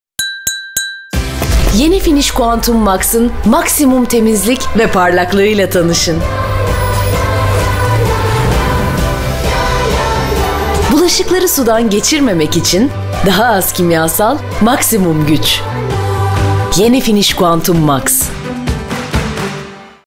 Finish | Yeni Finish Quantum Max ile
Tek bir damla su bile yarınlarımız için çok kıymetli. Bulaşıkları sudan geçirmeyin, sıyırın yeter. Gerisini Yeni Finish Quantum Max halleder. Bulaşıkları tertemiz yapmaya da, yarının suyunu korumaya da